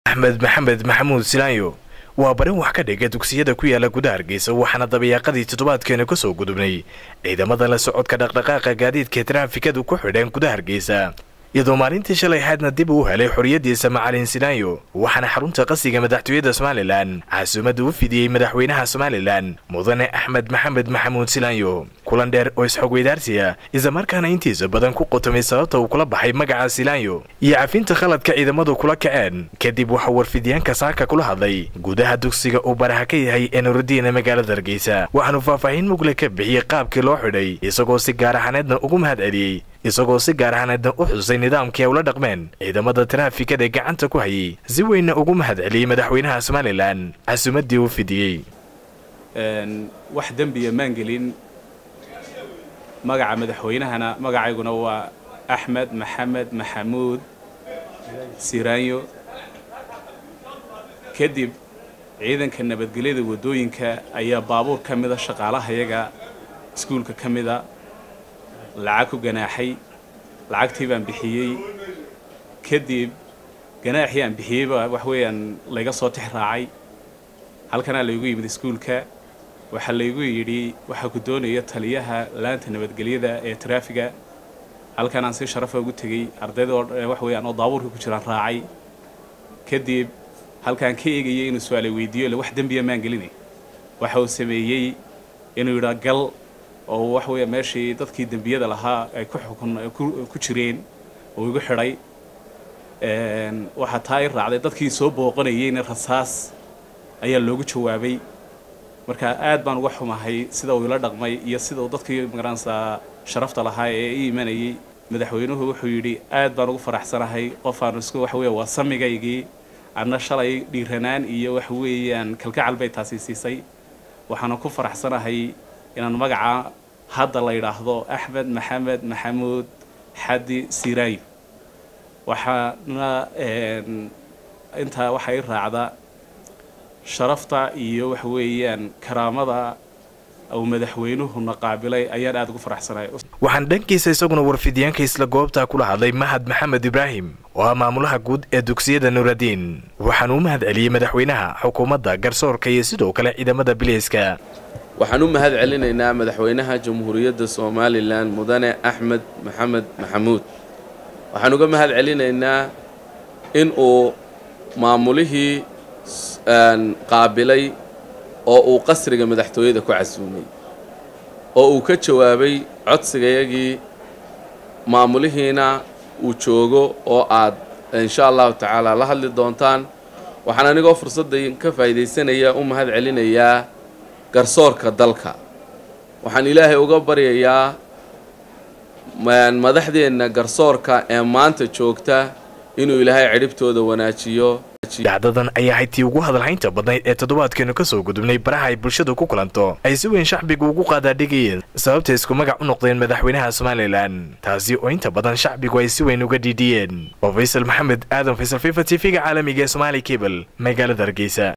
Warka Macalin Siiraanyo Oo Xoriyadiisa Dib Uheley Kana Waramay Siidii Loola Dhaqmay
Weriye Faysal Fifa Somali Cable